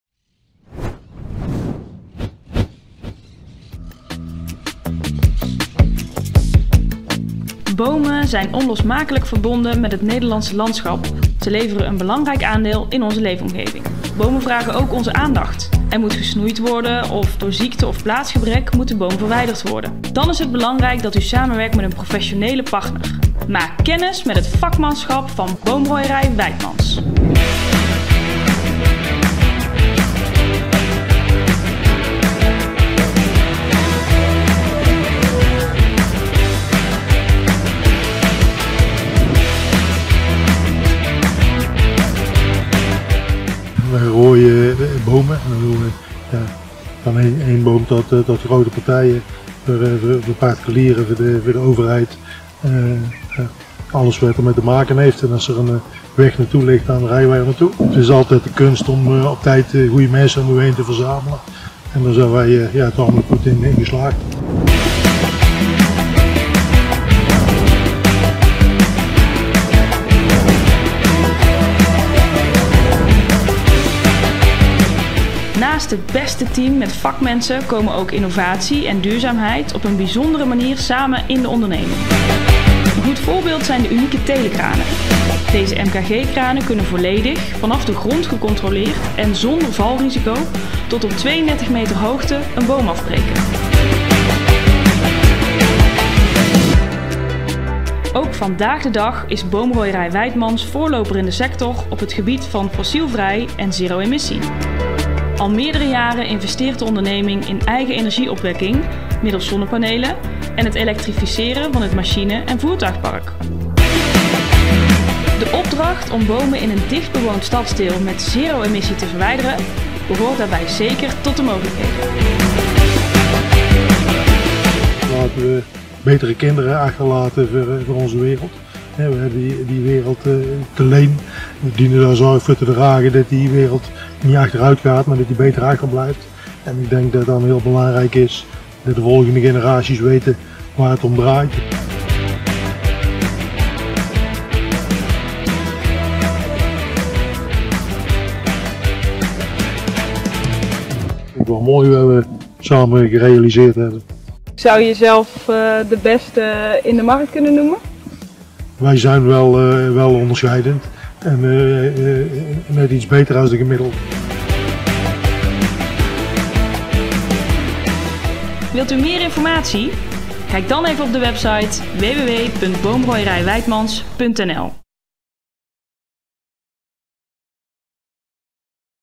Boomrooierij Weijtmans Bedrijfspresentatie
Boomrooierij Weijtmans is dé specialist voor bomen rooien, snoeien en nog veel meer. Wij zijn boomspecialist en houden ons naast het rooien van bomen bezig met het complete beheer en onderhoud van uw bomen en bossen. Gaat het om het vakkundig rooien van een boom in de tuin van een particuliere opdrachtgever; of de uitvoering van een bomenproject of beheerplan voor bedrijf of overheid. Bij Boomrooierij Weijtmans is uw opdracht altijd in ervaren en zeer vakkundige handen.